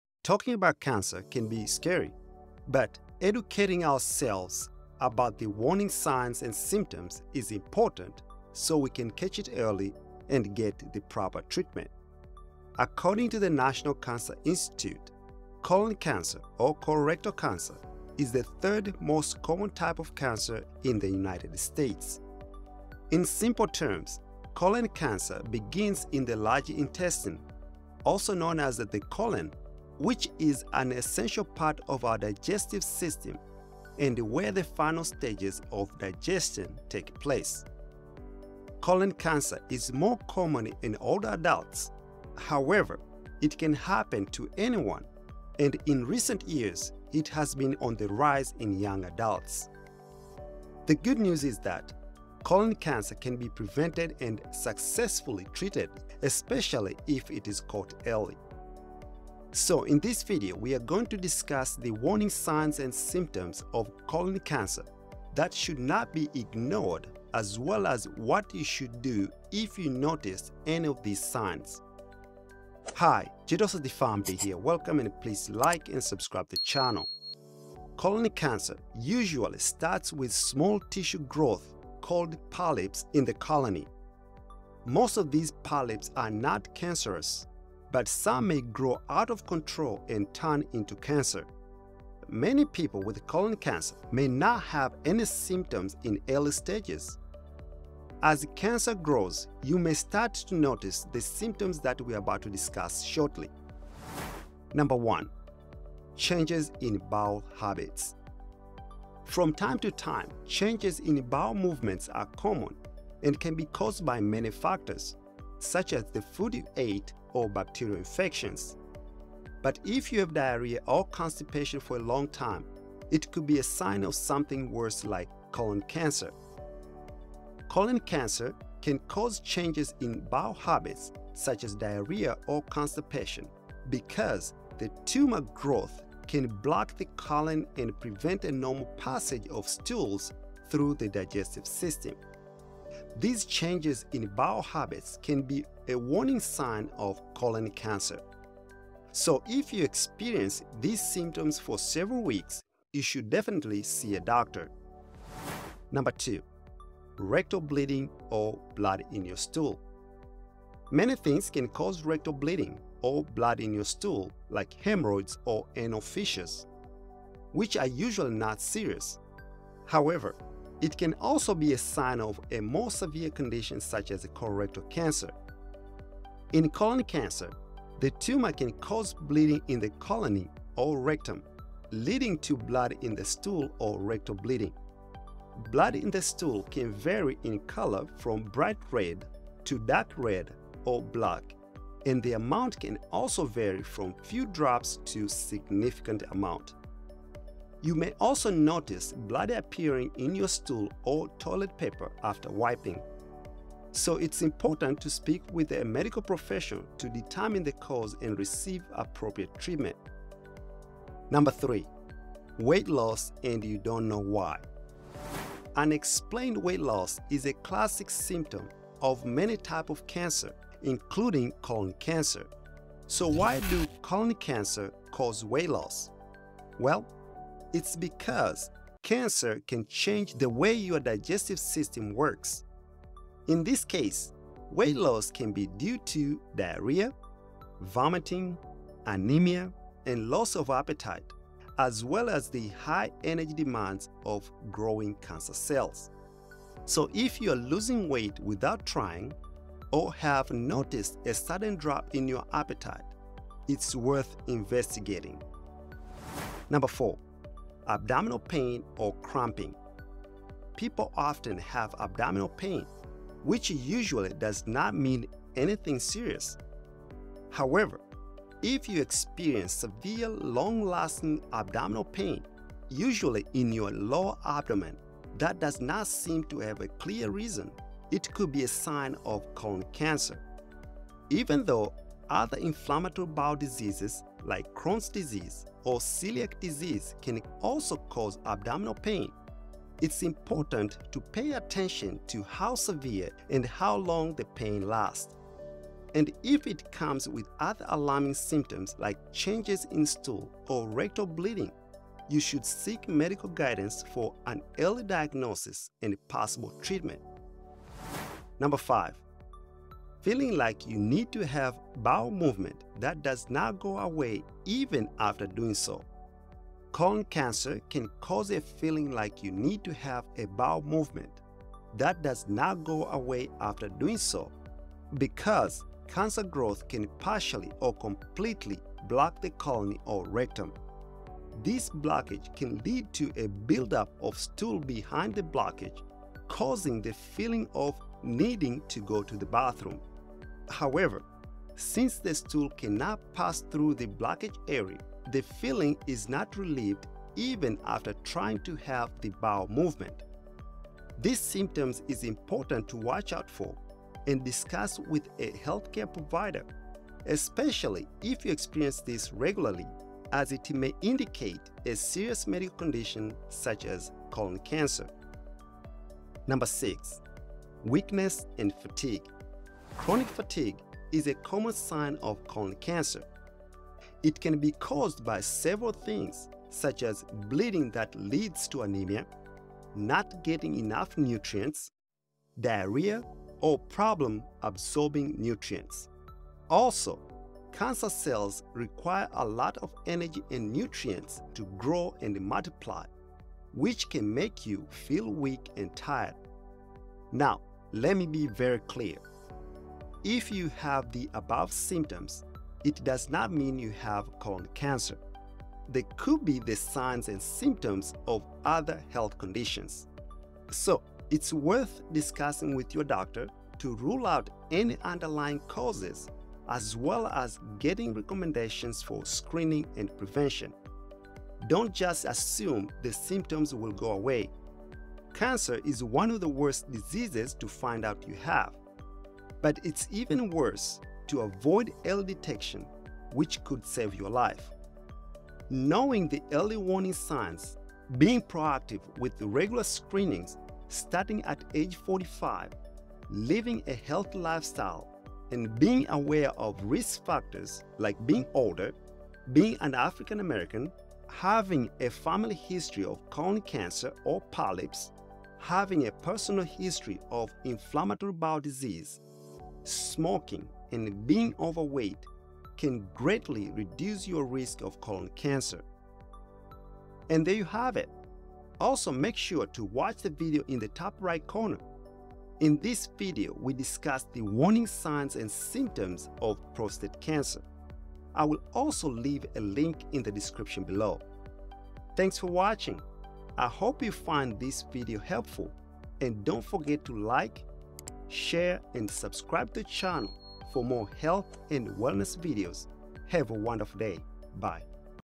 Colon Cancer Warning Signs and Symptoms | You Should Not Ignore Them
Colon cancer warning signs and symptoms. In this video, I will share the signs and symptoms of colon cancer, also known as colorectal cancer, that you should ignore. This way, you can take the necessary steps to seek early treatment if needed.

Summary:
Colon cancer, or colorectal cancer, is the third most common type of cancer in the United States. It begins in the large intestine, also known as the colon, which is an essential part of our digestive system and where the final stages of digestion take place. Colon cancer is more common in older adults; however, it can happen to anyone, and in recent years, it has been on the rise in young adults. The good news is that colon cancer can be prevented and successfully treated, especially if it is caught early.
Colon cancer usually starts with small tissue growths called polyps in the colon. Most of these polyps are not cancerous, but some may grow out of control and turn into cancer. Many people with colon cancer may not have any symptoms in the early stages. As cancer grows, you may start to notice the symptoms that we are about to discuss shortly.

Here are some common early signs and symptoms of colon cancer that should be aware of:
0:00- Intro: What is a Colon Cancer.
1:18- Colon Cancer Signs and Symptoms
1:45- Changes in digestive habits
2:37- Blood in Your Stool
3:32- Weight Loss and You Don’t Know Why
4:17- Abdominal (Belly) pain or Cramping
5:10- feeling like you have to go to the bathroom, but it doesn't go away after you do.
6:08- Weakness and Fatigue
7:53- Also Watch Prostate cancer signs and symptoms

I hope you find this video helpful in better understanding the common early warning signs and symptoms of colon cancer.